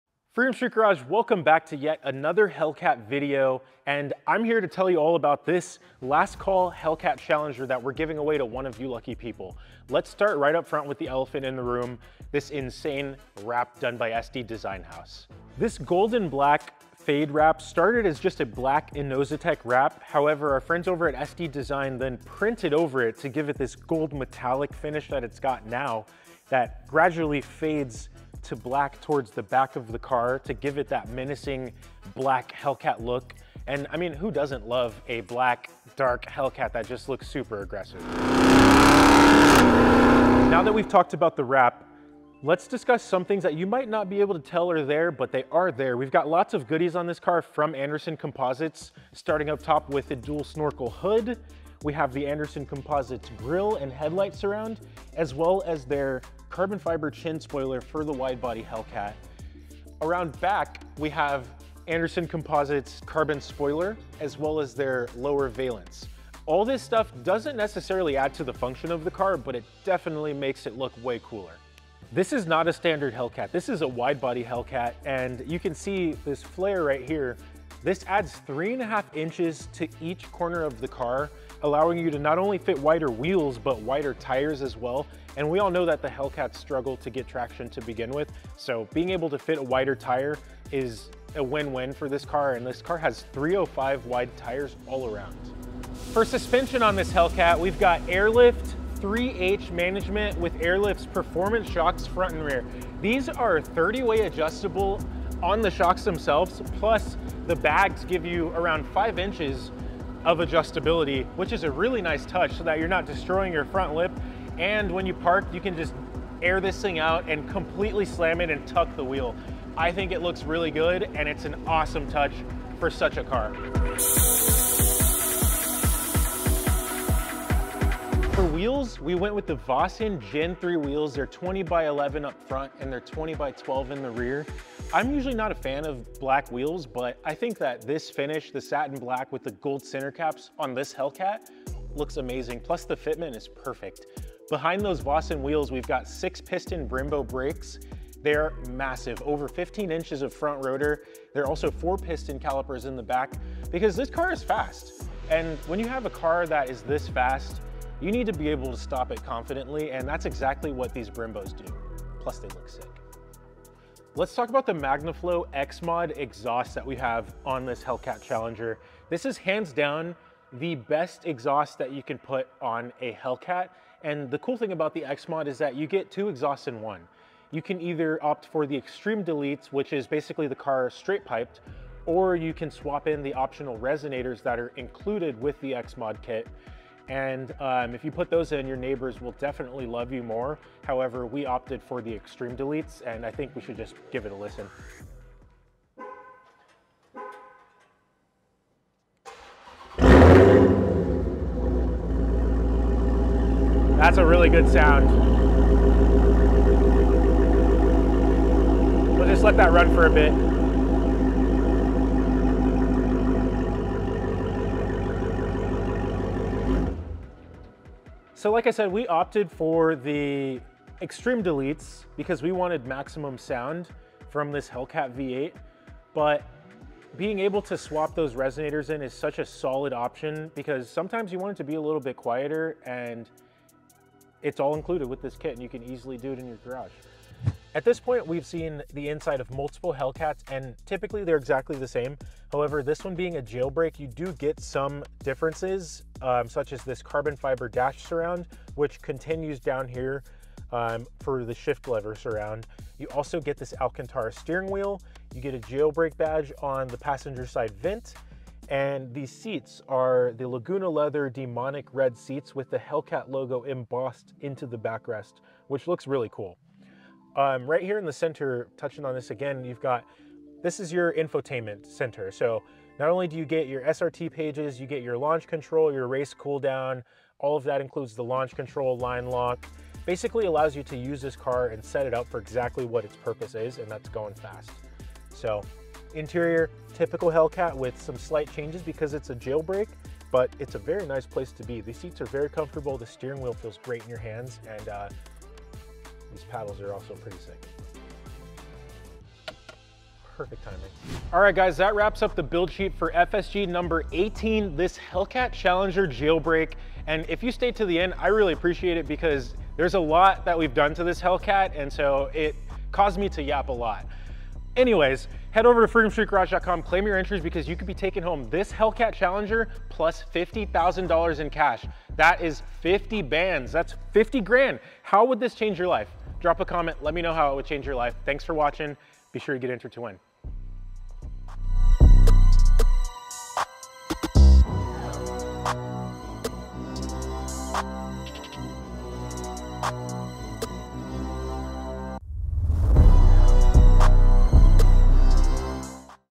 We BAGGED & Modded this Challenger Hellcat Widebody Jailbreak - FSG18 Buildsheet
Many of you may recall FSG 15, this exact 2023 Dodge Challenger Hellcat Widebody Jailbreak Last Call was given away last year.  Our 15th winner chose the cash alternative. So we brought back this 717HP beast as FSG18, this time with tons of insane modifications! See the full list of mods below:

✅ Airlift Performance Bags
✅ Inozetek Gold and Black custom wrap
✅ Magnaflow XMOD Cat-back Exhaust
✅ Anderson Composites Carbon Hood, Front chin spoiler, grill surround, rear valance, and spoiler
✅ Vossen Gen-03 Wheels in Satin Black with Imperial Gold Center Caps, 20x11 front, 20x12 rear
✅ CORSA Carbon Fiber Air Intake w/ Oiled Filter
✅ Window Tint
✅ Blacked out reflectors